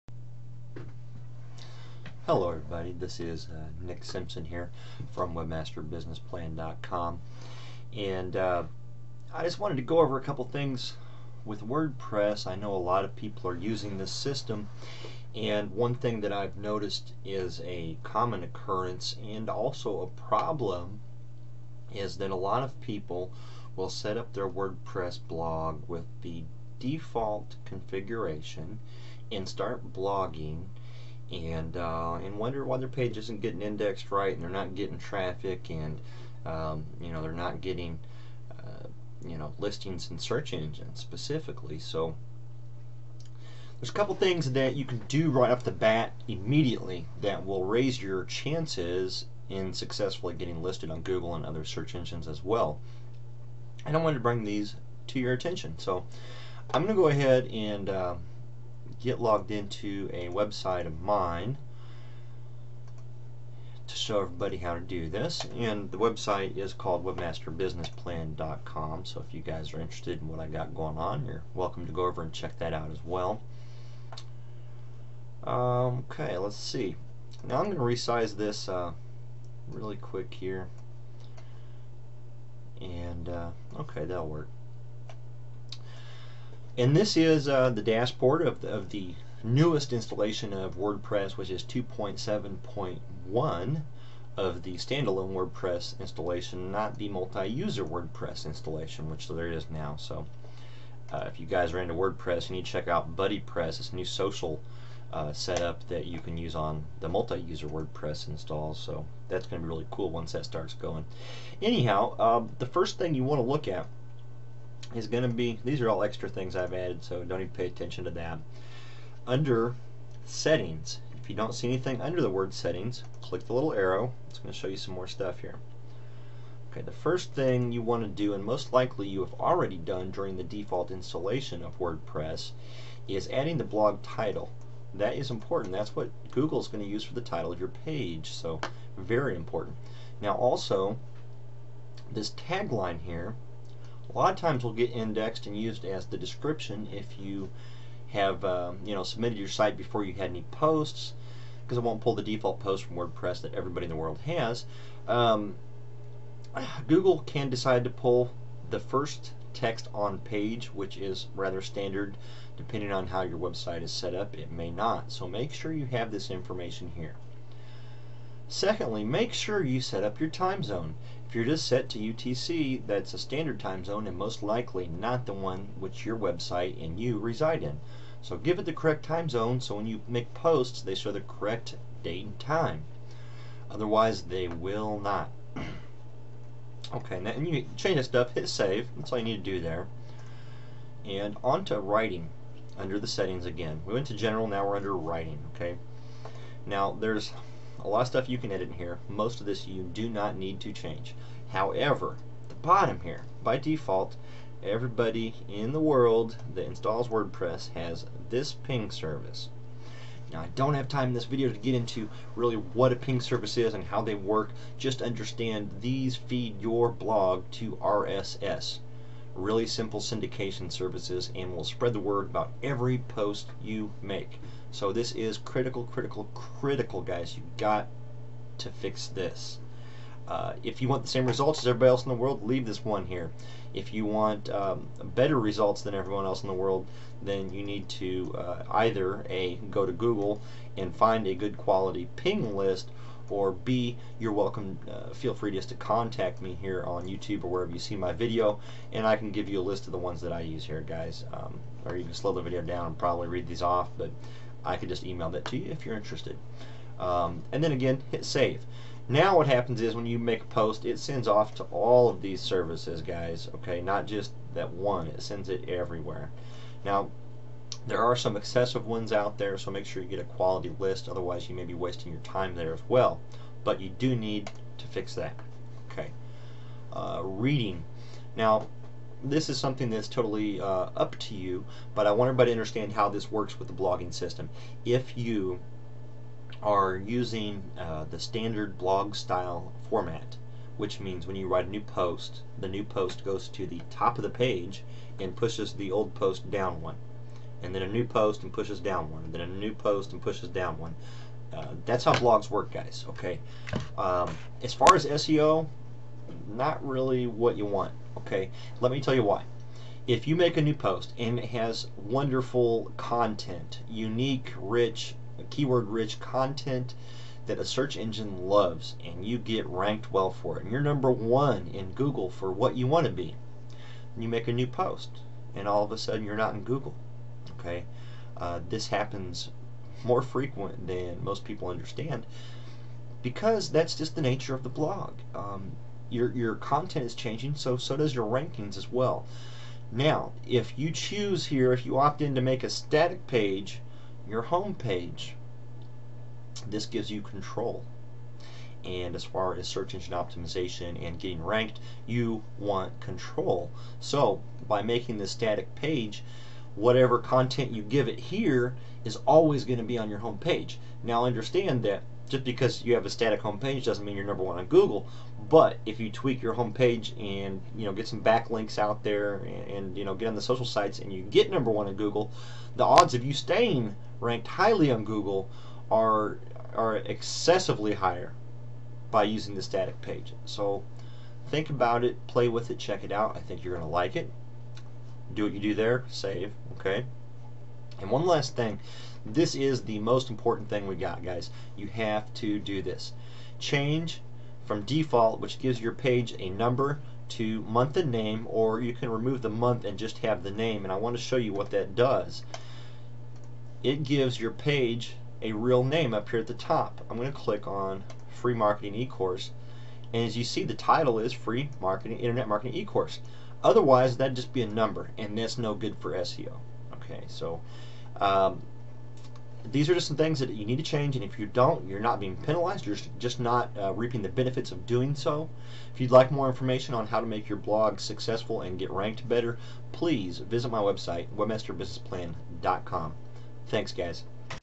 Free Wordpress SEO Tips By Nick Simpson
There are many things that wordpress does automatically that are wonderful for search engine optimization, but there are also some things it does not do that you really want to correct before your site get indexed by google. It just takes a few minutes to get your wordpress blog seo ready, so watch the video and enjoy!

To Success,

Nick Simpson